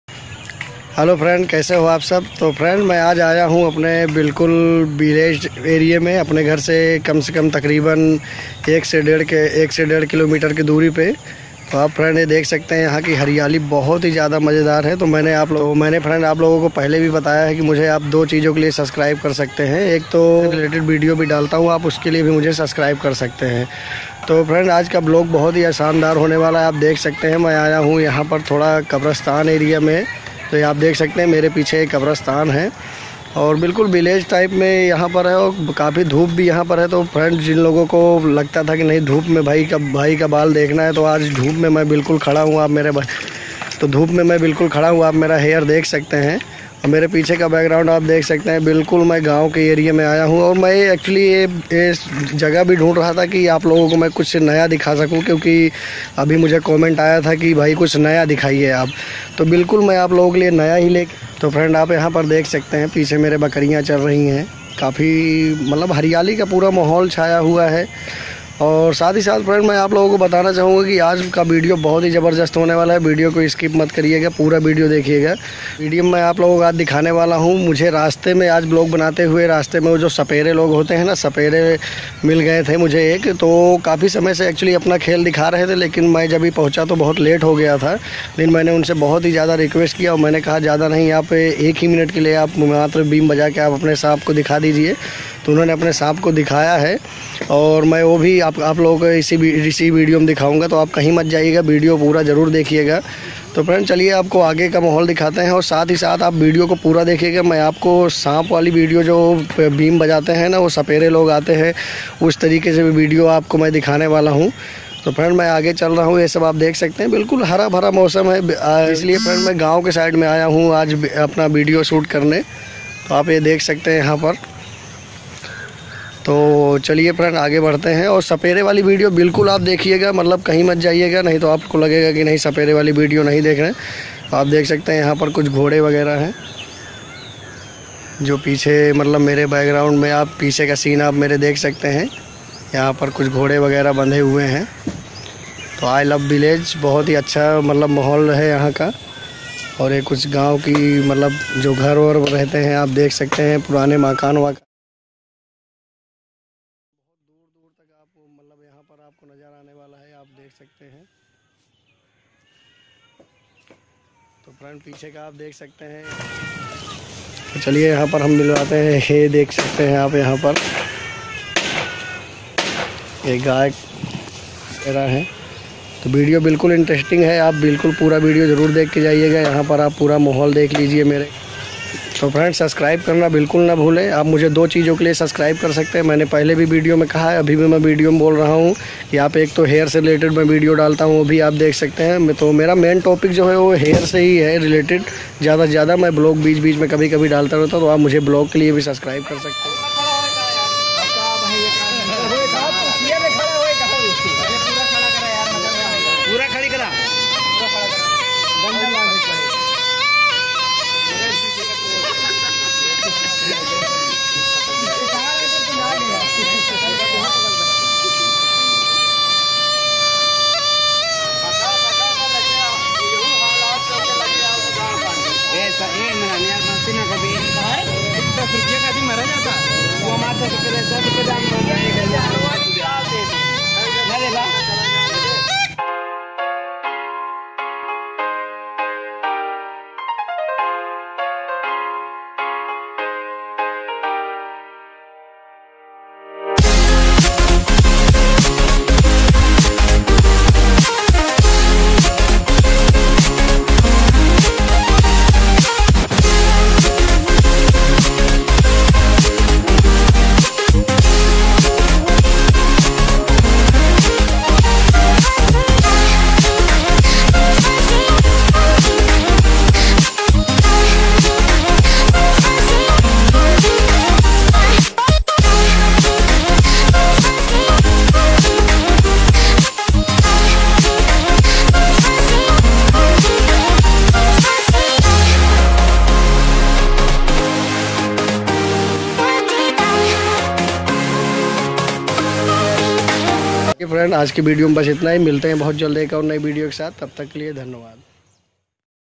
धूप में बाल कम दिखते हैं | balayam hair growth | sun light hair density | i love village
Hello friend aaj ke video me maine apana
 hair density ke bare me bataya hai sun
 light me mere bal kaise dikhate hai dhup
 me balo ko dikhaya hu or sath hi maine
 apane village ke bare me bataya hai
video pura dekhe dhnyavad

Balayam experience

Balayam experience with hair care

I love village

Balayam hair growth

Sun light hair density

Dhup me bal kam ikhate hai